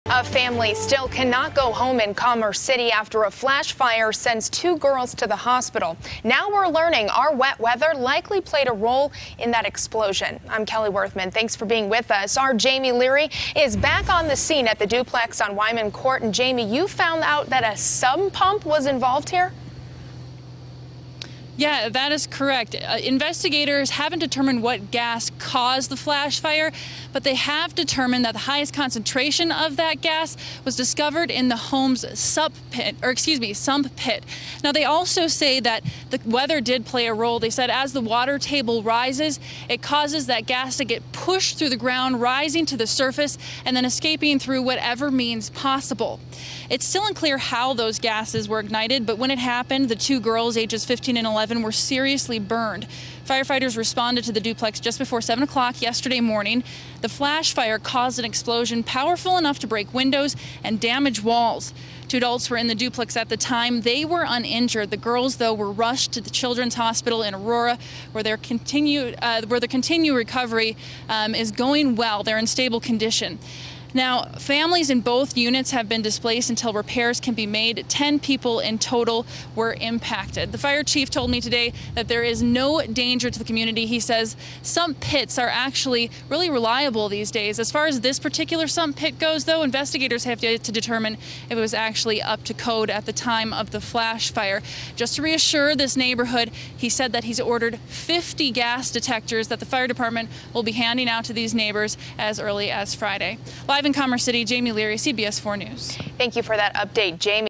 Family Remains Out Of Home After Flash Fire
2 girls were severely burned in the explosion in Commerce City.